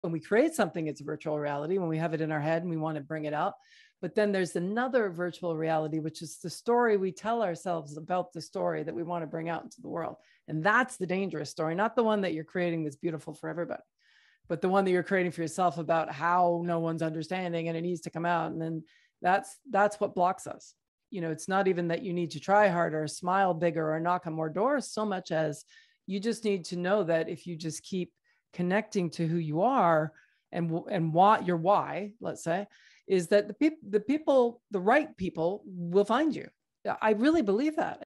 We Create Our Own Virtual Reality. What stories have you created that aren't serving you?
An excerpt from a coaching conversation with Lisa Hopkins. As storytellers, creative people imagine and bring to life all sorts of imagined stories. But what of the stories that we create about ourselves? We are living in a kind of virtual reality that we create ourselves - in our heads. What would be different if we could rewrite THOSE stories?